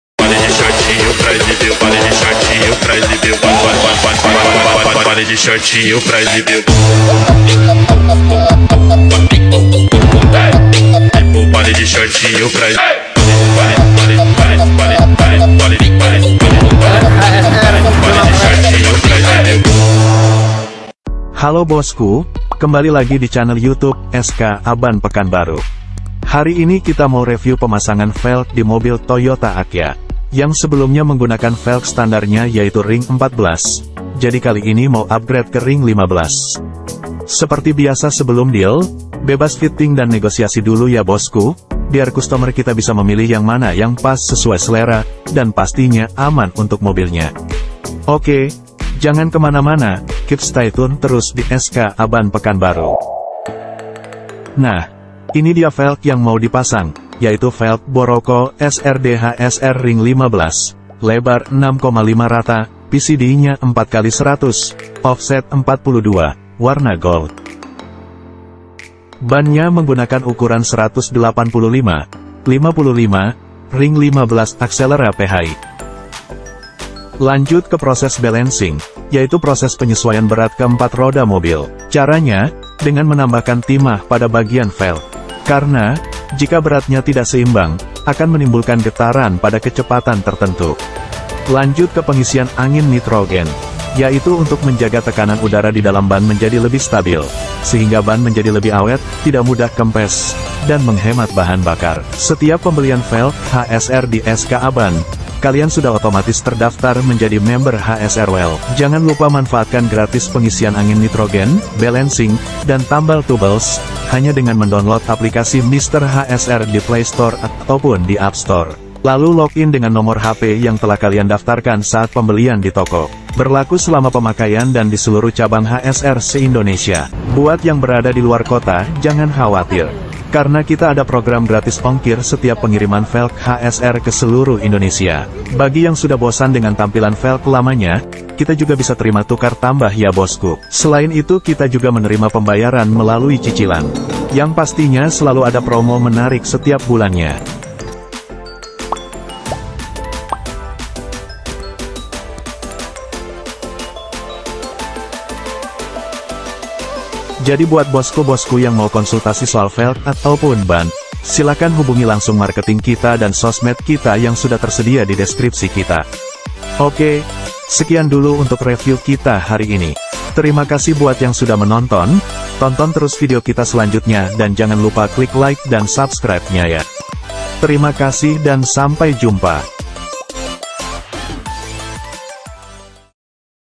Toyota Agya upgrade dari Velg Standar ke Velg HSR Boroko SRD Ring 15
*mohon maaf jika ada kesalahan kata dan ketidak sempurnaan di video.. masih pemula, harap dimaklumi yaa.. semoga kedepan bisa lebih baik. support terus skaban.. thankyou ^_^